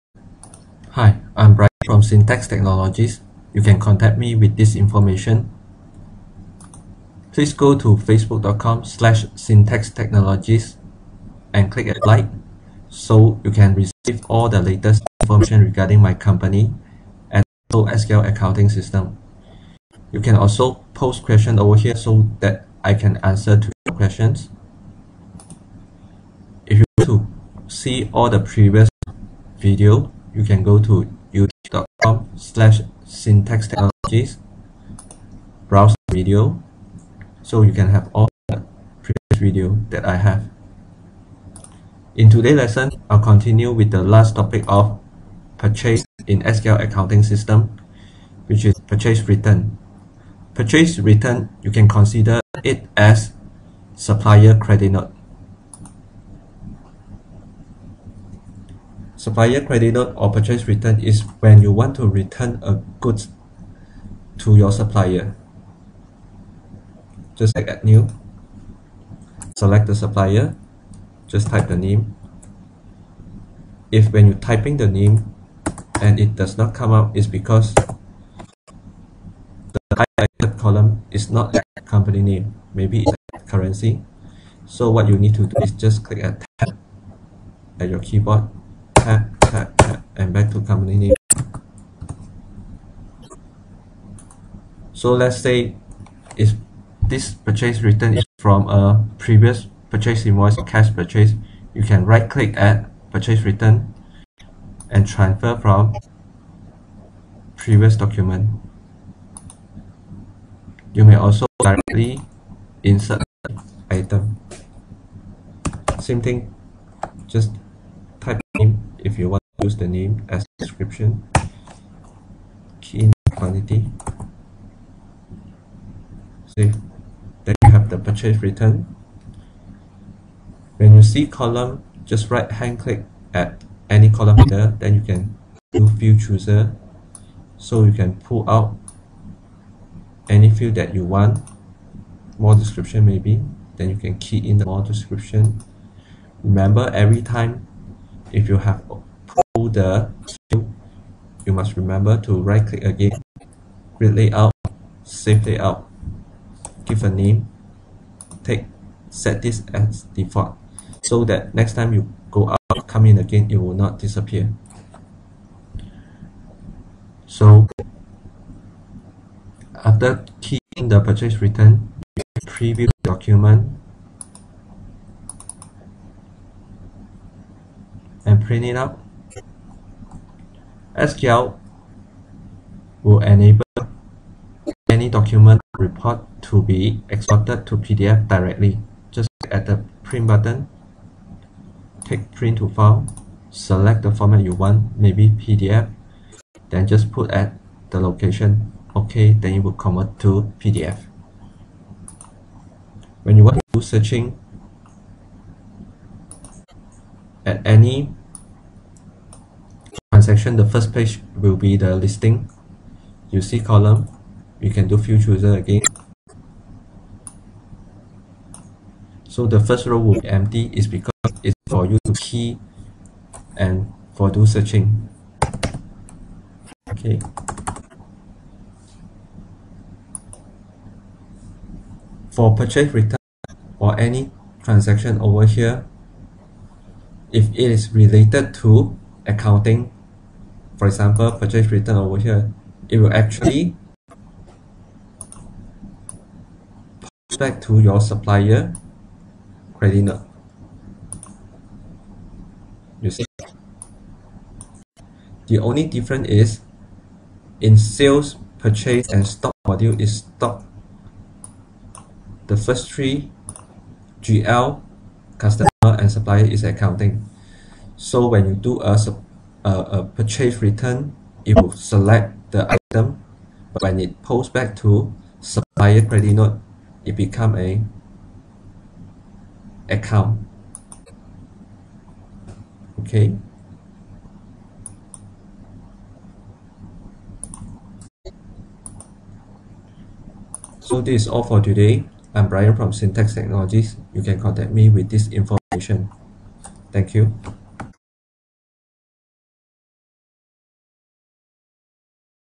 A038 Purchase Returned - SQL Accounting Software
This video shows you how to do Purchase Returned In Purchase Module.